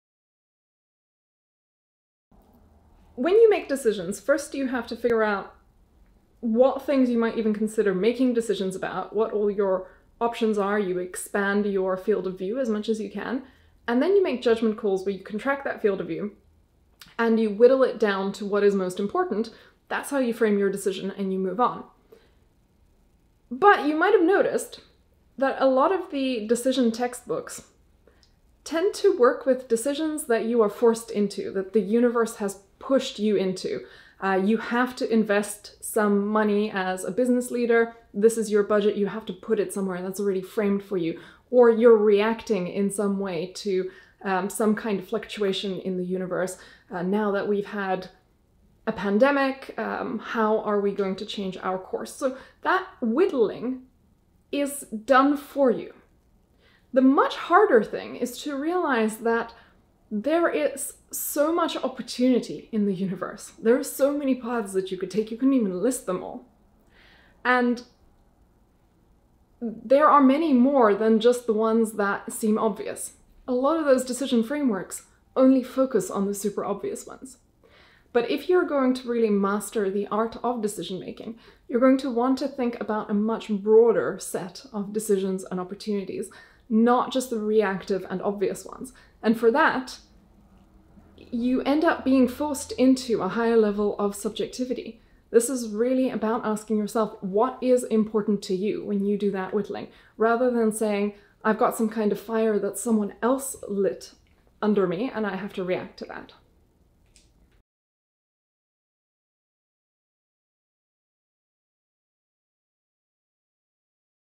How to make better decisions
If you don't take the time to set your priorities, someone else will set them for you. So here's a simple trick to improve your decision quality: expand-then-contract. Make sure you separate your decision framing process into two separate phases... The video explains.

Mental expansion and contraction in decision-making:
Phase 1 - Expand your field of view: Identify as many options as possible.
Phase 2 - Contract your field of view: Whittle down your options based on what's important to you.

This will help you strike a good balance between proactive and reactive decision-making and prevent you from getting overwhelmed by all your options.

(If you came here looking for data science content, did you read between the lines? The best thing for helping a decision-maker expand their options is analytics, while the best things for helping them contract their options into clear decision criteria is fact-finding, statistics, and classical decision science. Splitting your data and approaching decision-making in two phases is your secret weapon for maximizing your decision quality!)

If you found this useful, the best way to say thank you is by sharing it.